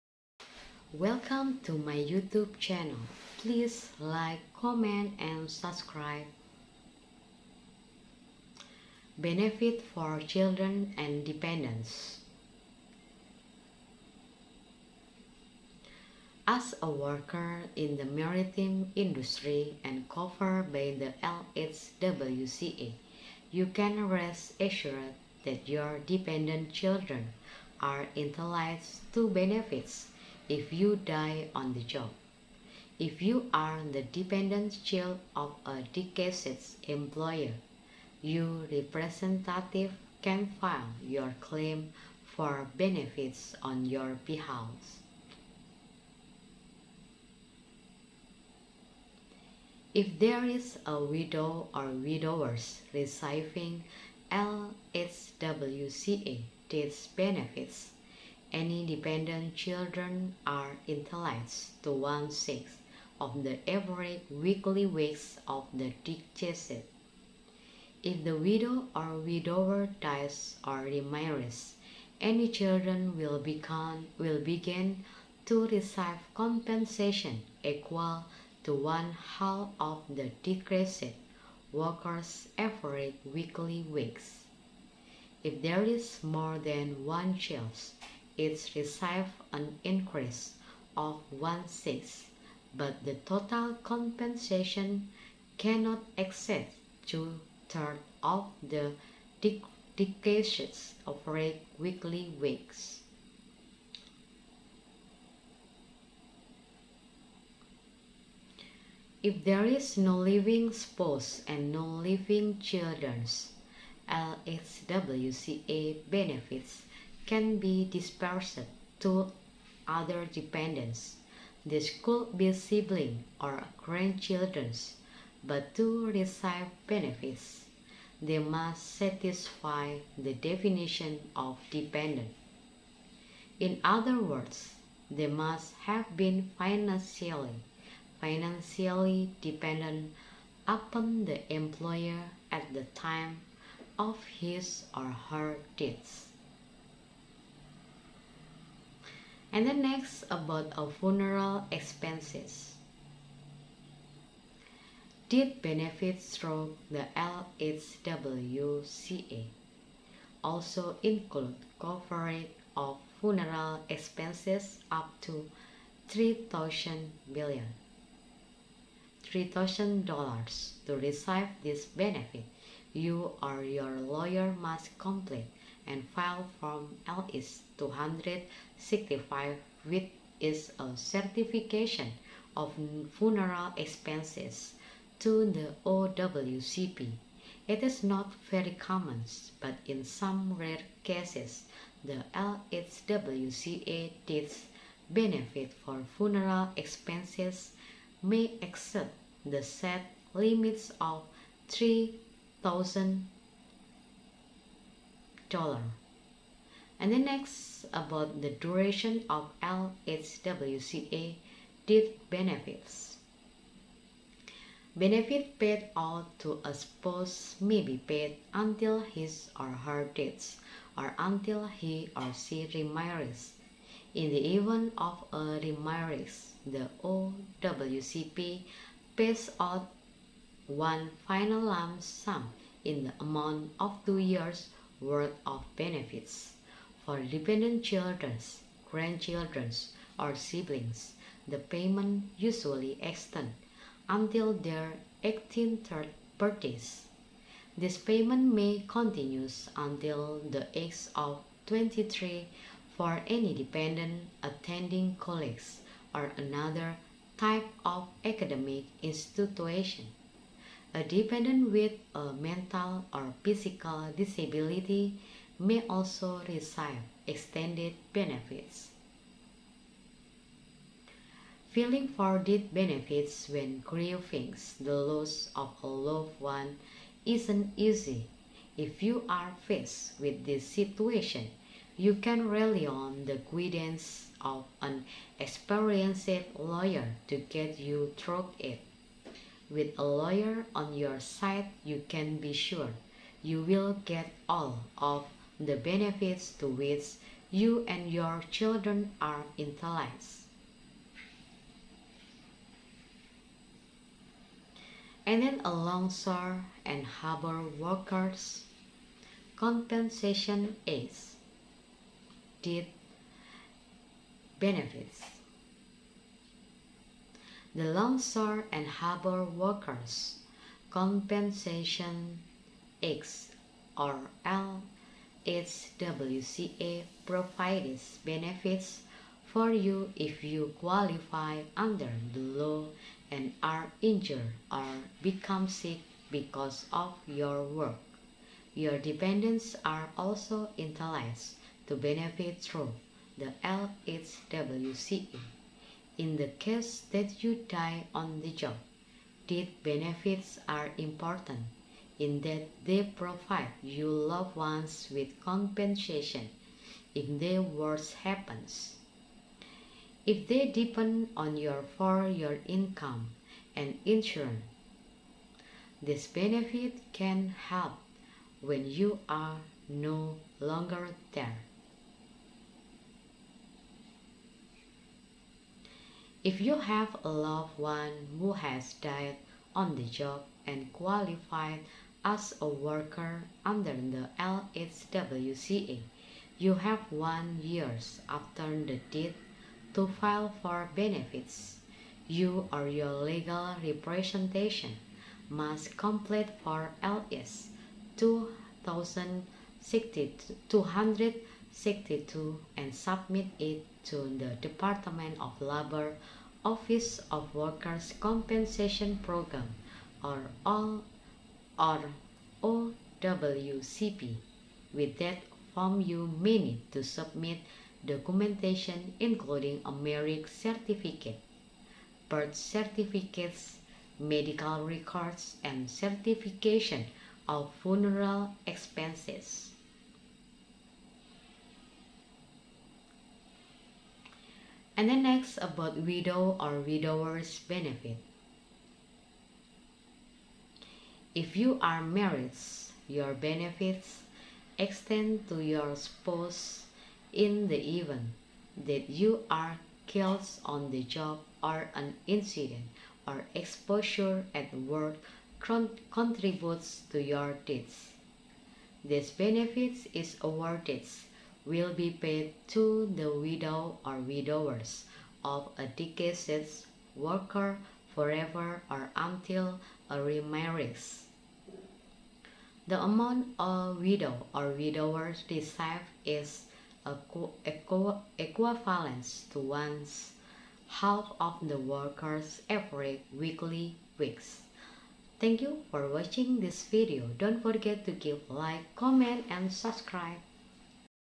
Your children are entitled to benefits if you die as a worker in the maritime industry
As a worker in the maritime industry and covered by the LHWCA you can rest assured that your dependent children are entitled to benefits if you die on the job. If you are the dependent child of a deceased employee, your representative can file your claim for benefits on your behalf.
If there is a widow or widower receiving LHWCA death benefits, any dependent children are entitled to one-sixth of the average weekly wage of the deceased. If the widow or widower dies or remarries, any children will begin to receive compensation equal to one-half of the deceased worker’s average weekly wage. If there is more than one child, each receives an increase of one-sixth, but the total compensation cannot exceed two-thirds of the deceased’s average weekly wage.
If there is no living spouse and no living children, LHWCA benefits can be dispersed to other dependents. These could be siblings or grandchildren, but to receive benefits they must satisfy the definition of dependent. In other words, they must have been financially dependent upon the employee at the time of his or her death.